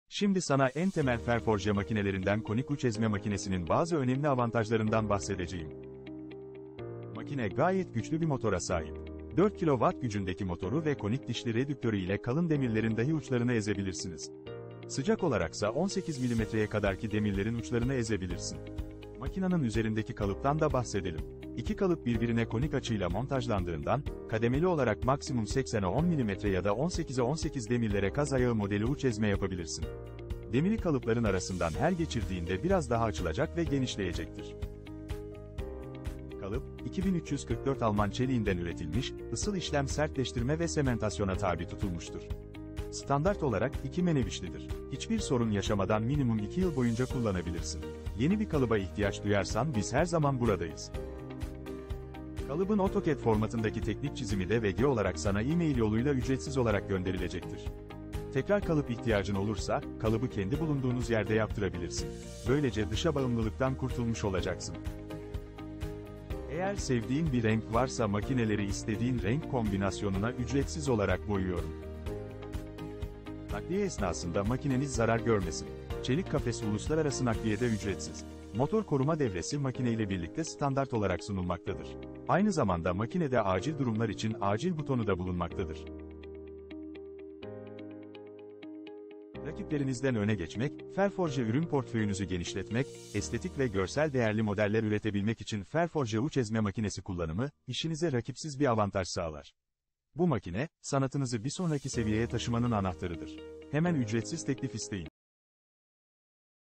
Ferforje Uç Ezme Makinesi (EF02)
Ferforje projelerinizi mükemmel uçlarla tamamlamak için ideal bir araç arıyorsanız, Ferforje Uç Ezme Makinesi (EF02) tam size göre. Bu makine, demir işçiliğinde işlerinizi kolaylaştırır ve sonuçları profesyonelce şekillendirir.